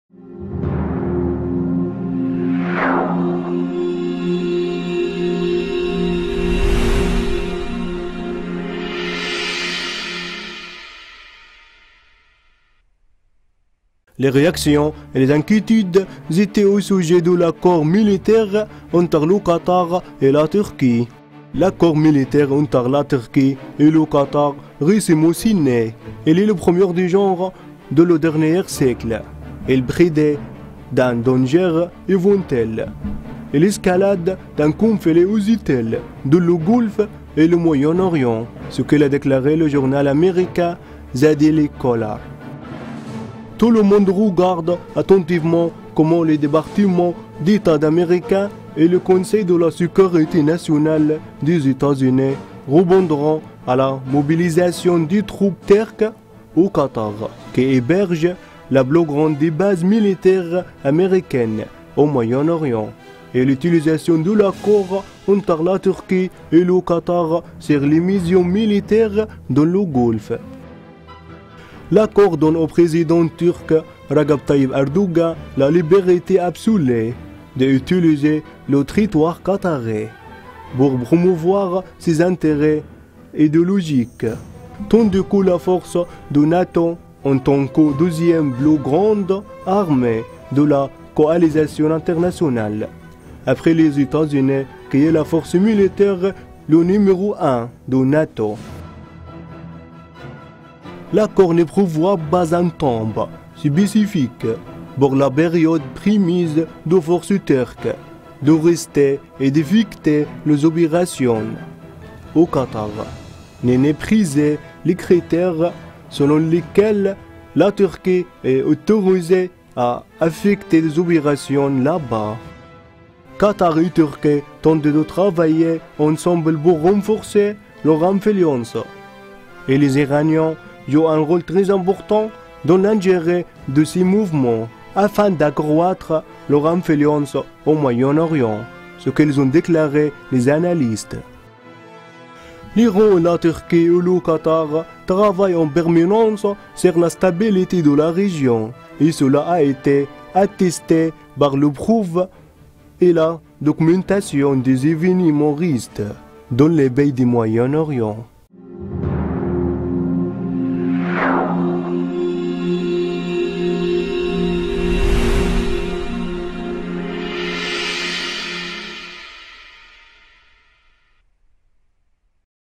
Les réactions et les inquiétudes étaient au sujet de l'accord militaire entre le Qatar et la Turquie
«L'accord militaire entre la Turquie et le Qatar récemment signé est le premier du genre dans le dernier siècle, il prédit d'un danger éventuel et l'escalade d'un conflit hostile dans le golfe et le Moyen-Orient», ce qu'il a déclaré le journal Américain «The Daily Caller».

Tout le monde regarde attentivement comment le département d'État Américain et le Conseil de la sécurité nationale des États-Unis répondront à la mobilisation des troupes turques au Qatar, qui héberge la plus grande base militaire Américaine au Moyen-Orient et l'utilisation de l'accord entre la Turquie et le Qatar sur les missions militaires dans le Golfe.

L'accord donne au président turc Recep Tayyip Erdogan la liberté absolue d'utiliser le territoire Qatari pour promouvoir ses intérêts idéologiques, tandis que la force de NATo en tant que deuxième plus grande armée de la Coalition internationale, après les États-Unis, qui est la force militaire le numéro un de NATo.
L'accord ne prévoit pas un temps spécifique pour la période permise de forces Turques de rester et d'effectuer leurs opérations au Qatar, ni ne précise les critères selon lesquels la Turquie est autorisée à effectuer des opérations là-bas.

«Qatar et la Turquie tentent de travailler ensemble pour renforcer leur influence et les Iraniens jouent un rôle dans l'ingénierie de ces mouvements, afin d'accroître leur influence au Moyen-Orient», ce qu'ils ont déclaré les analystes.

L'Iran, la Turquie et le Qatar travaillent en permanence sur la stabilité de la région et cela a été attesté par la preuve et la documentation des événements récents dans les pays du Moyen-Orient.